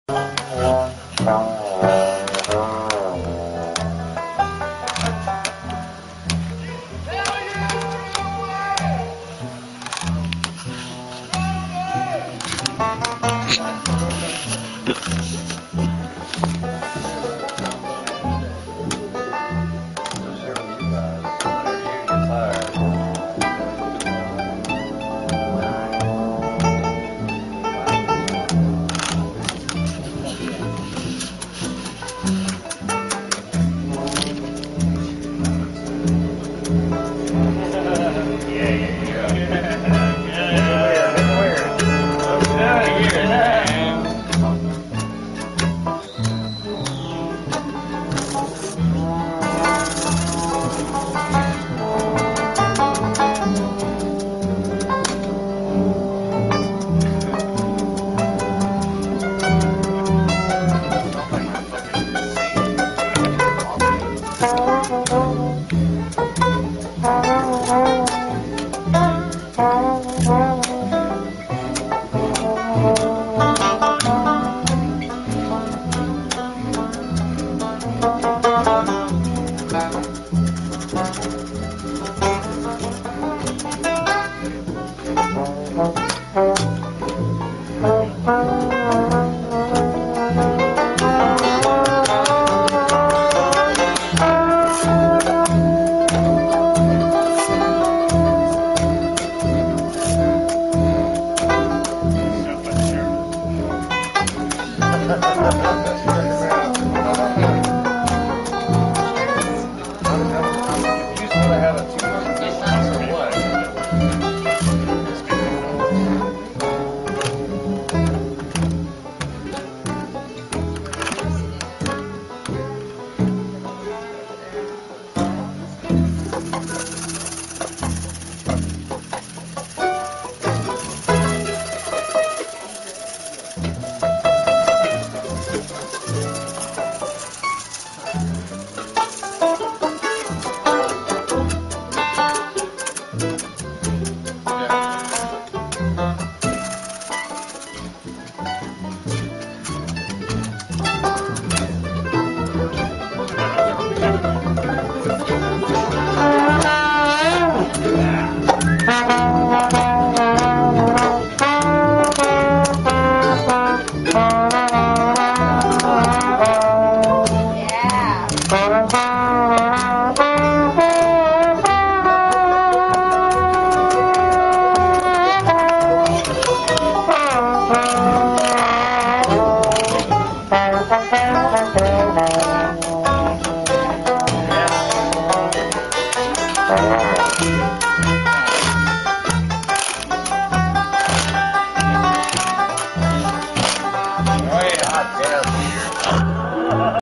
Jam Session at Winfield, too dark to see, but pretty sweet to hear.
It was so dark I can't even tell you who it was.  Tyler Grubb was there, obviously.